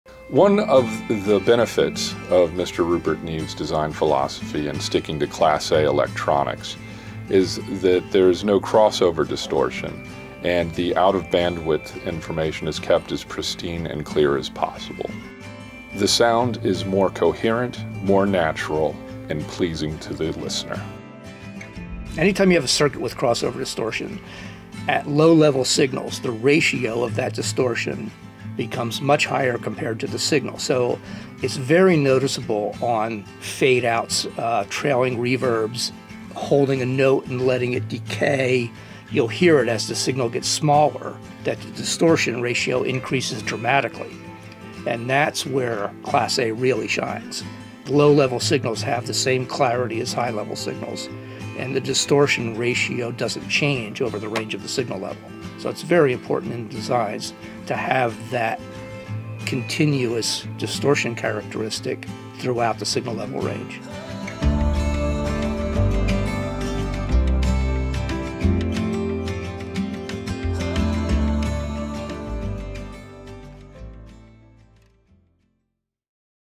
Design: Class-A Electronics
Learn why Class-A electronics are such an important element in the sound of a Rupert Neve design.